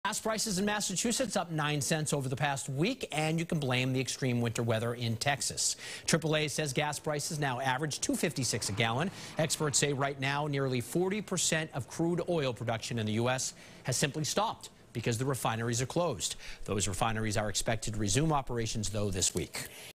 Massachusetts Gas Prices Spiking After Damaging Winter Weather
Refinery outages were reported in Texas after the extreme weather there.